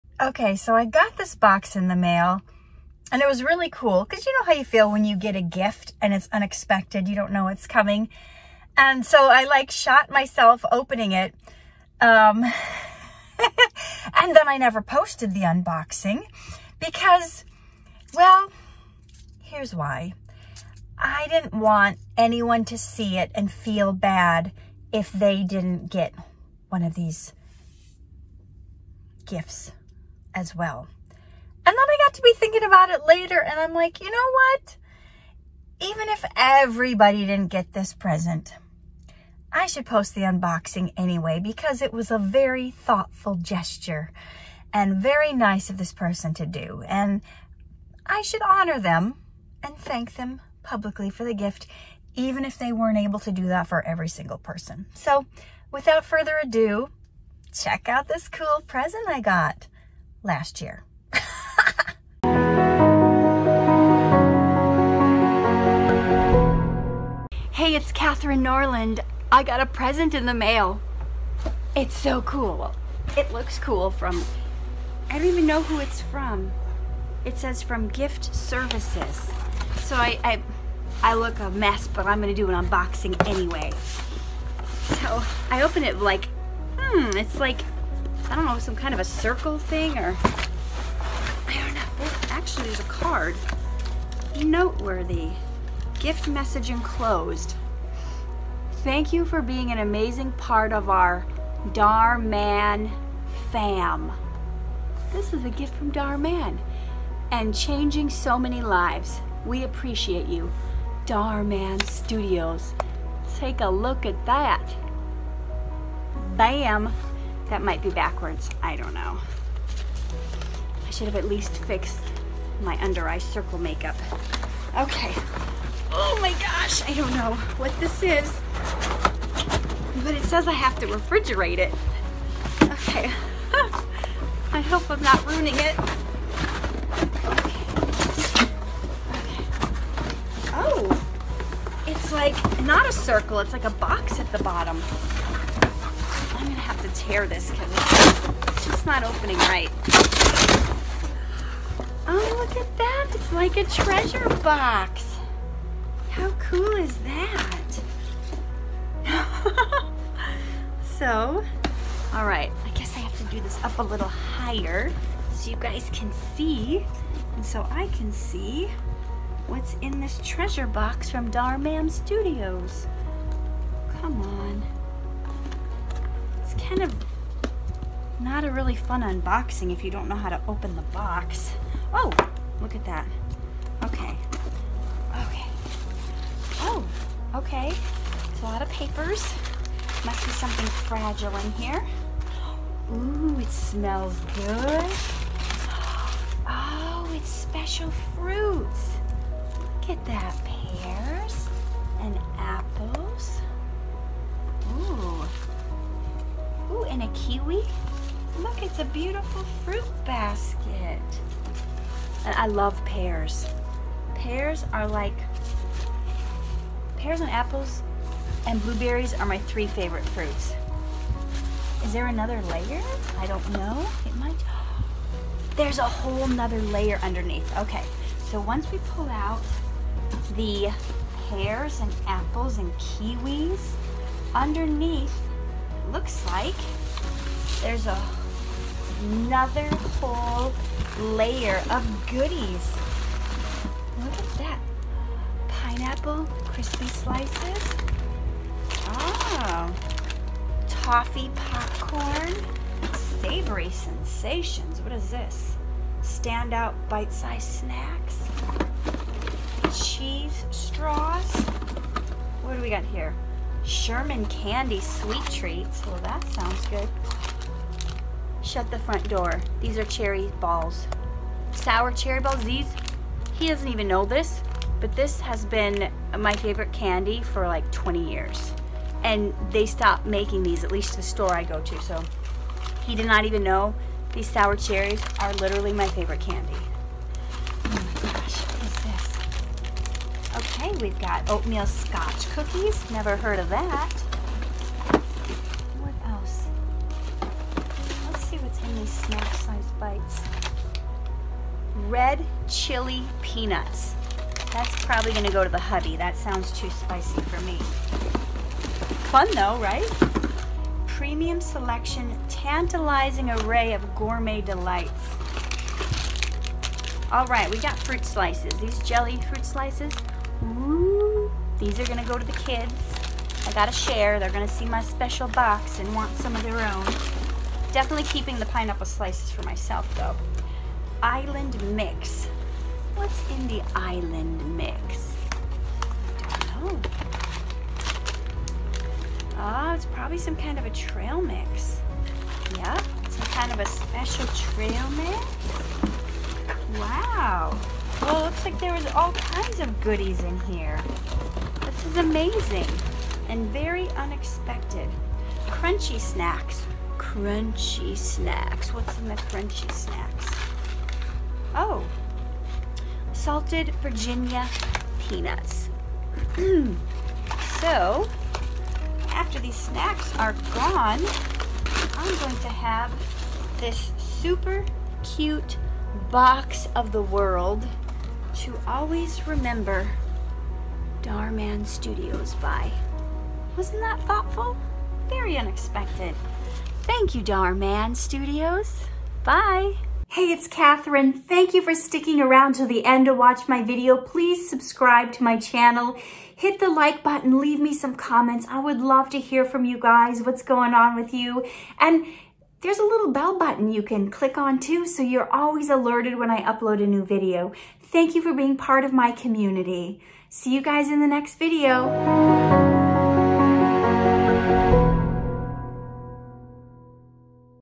Dhar Mann Gift Unboxing | Katherine Norland
Unboxing a special box from Dhar Mann Studios.